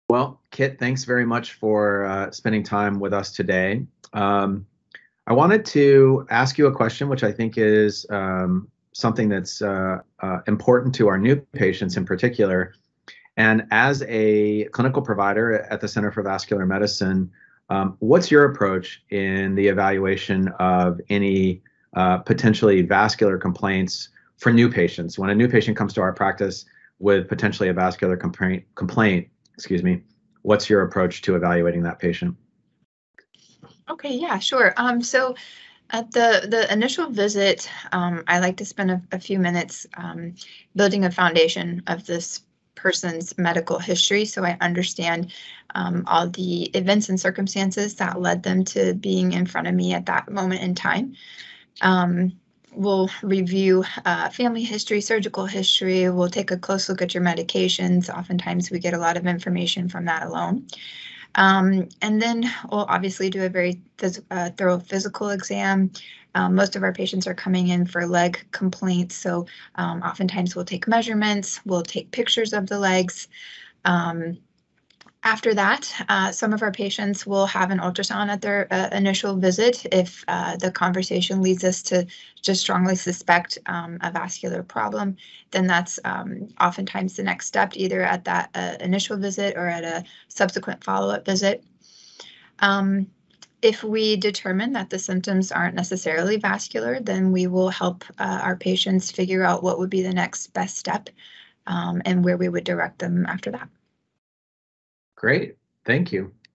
Krystal Alcott, Vascular Specialist, Explains New Patient's First Appointment.
What to expect when visiting a vascular specialist at the Center for Vascular Medicine?